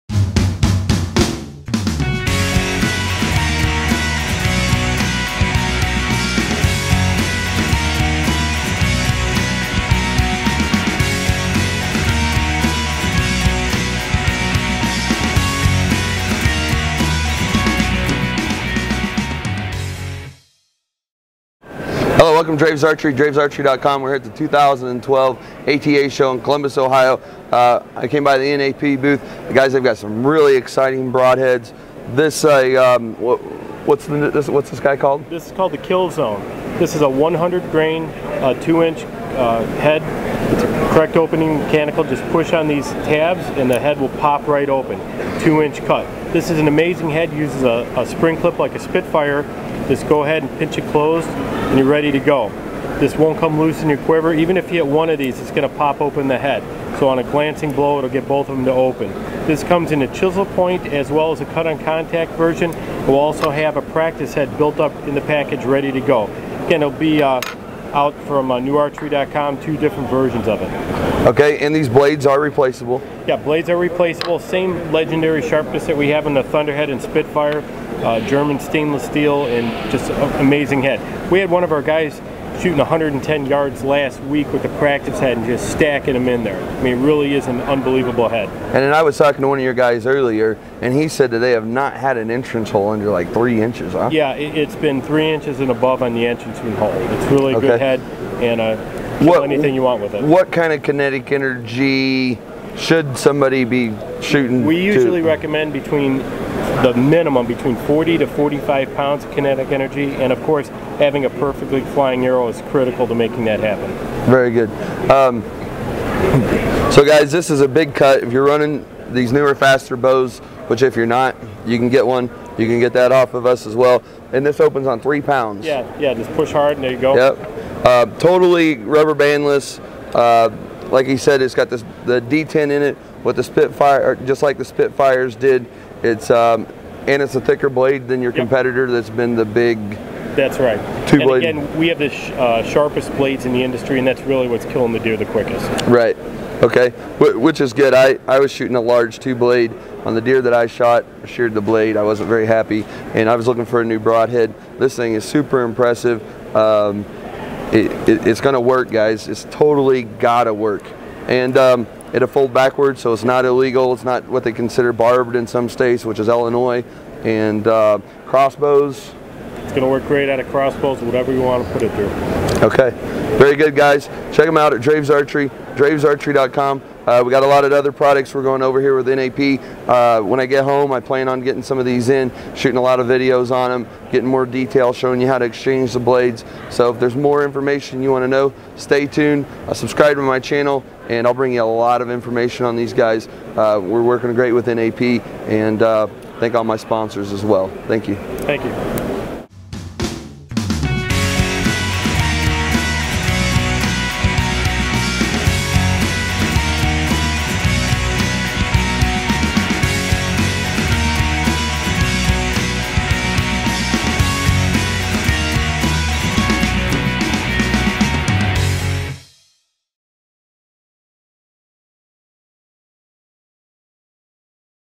New Archery Products NEW Killzone Mechanical Broadhead | 2" Cutting Diameter
The new Killzone is a 2 blade rear-deploying broadhead that packs a devastating 2" cutting diameter. The field-point accurate Killzone uses NAP's unique spring-clip design which ensures your blades won't open in flight or in your quiver. There are no o-rings or rubber bands to worry about.

The Killzone is available in three different models including a cut-on-contact tip, a bone-crushing Trophy Tip, or the Deep Six compatible model. No matter which version you choose, the Killzone ensures that you will always Hunt with Confidence.